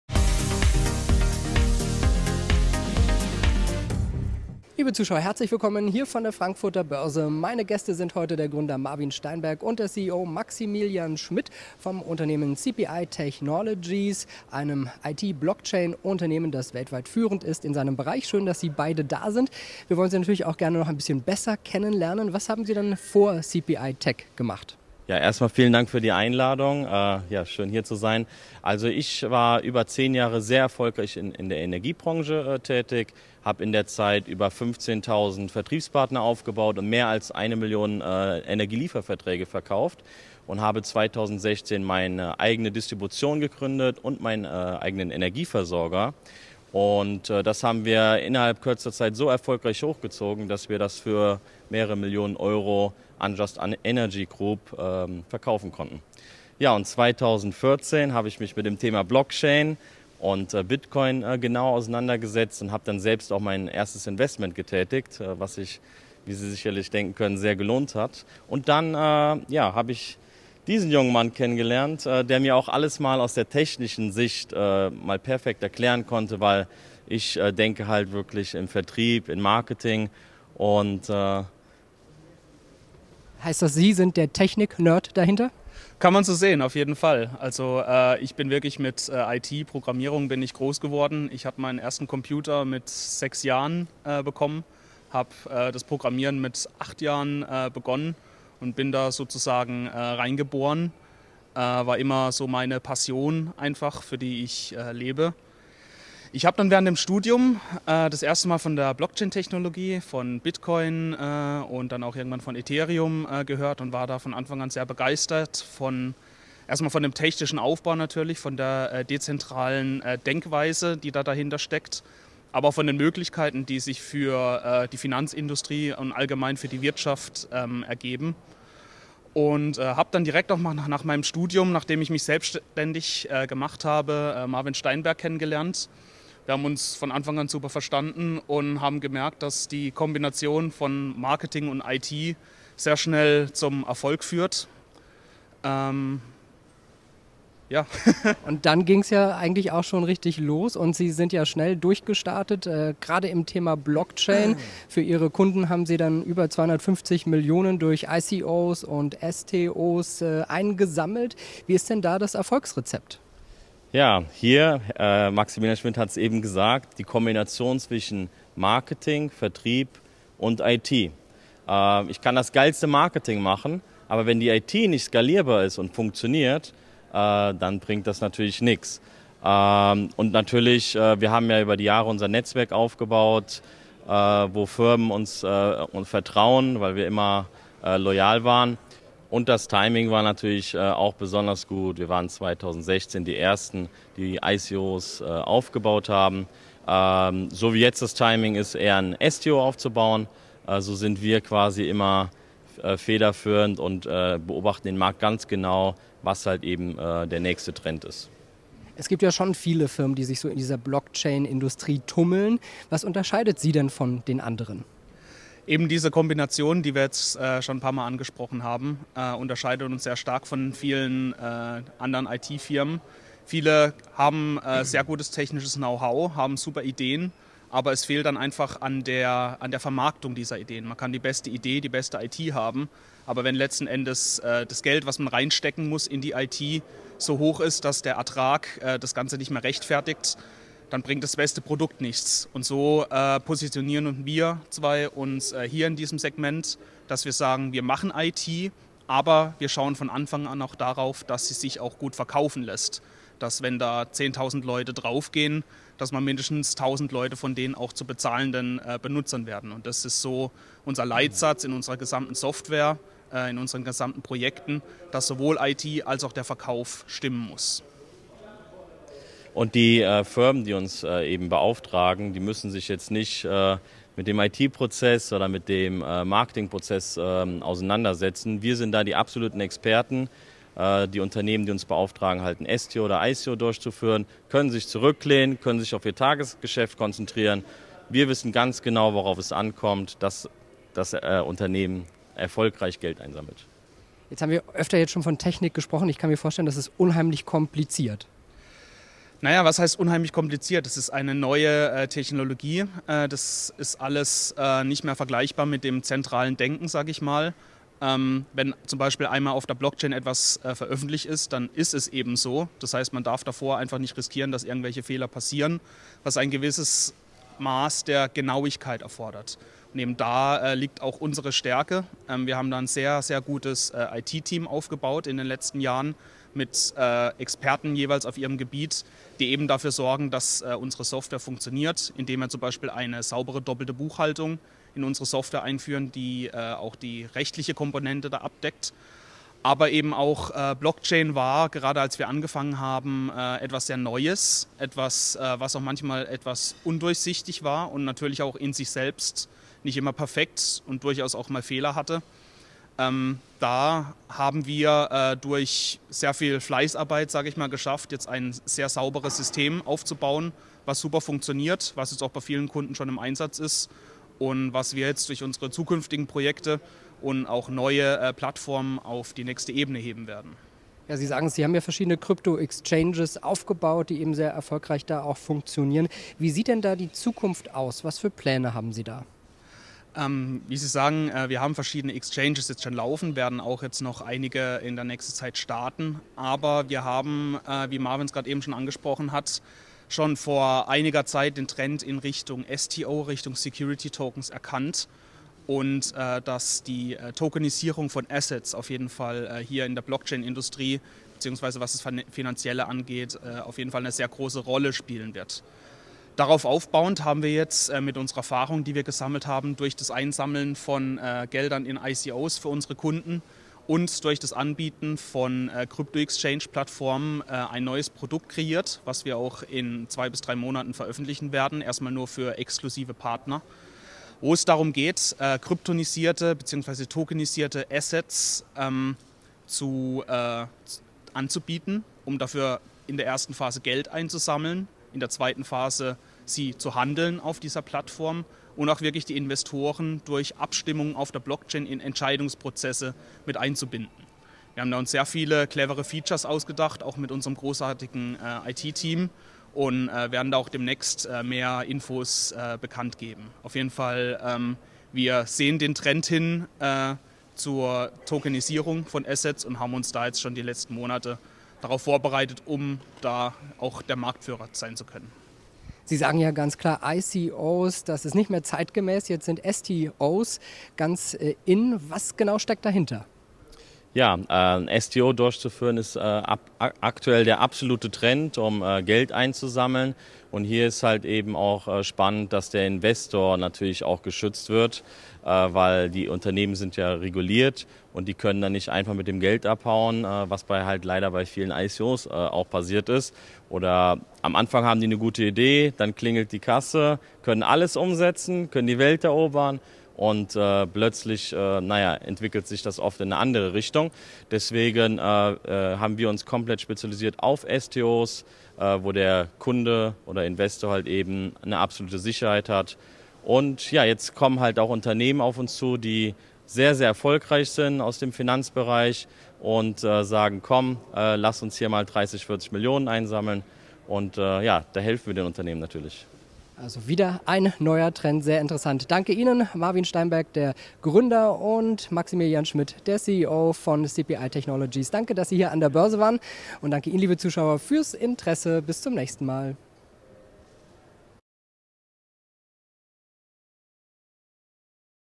Neuer Trend: CPI Technologies setzt auf Security Token Offerings (STOs)
"Security Token Offerings (STOs) durchzuführen ist aktuell der absolute Trend, um Geld einzusammeln. Und hier ist auch spannend, dass der Investor geschützt wird, weil die Unternehmen reguliert sind. Die können also nicht einfach mit dem Geld abhauen, wie es bei ICOs oft der Fall war", sagt Marvin Steinberg. Der Gründer von CPI Technologies setzt ganz klar auf diese Sicherheiten. CEO Maximilian Schmidt ergänzt: "Wir machen nicht nur die IT, sondern wir schauen von Anfang an auch darauf, dass es sich gut verkaufen lässt. Denn sowohl IT und Verkauf müssen stimmen."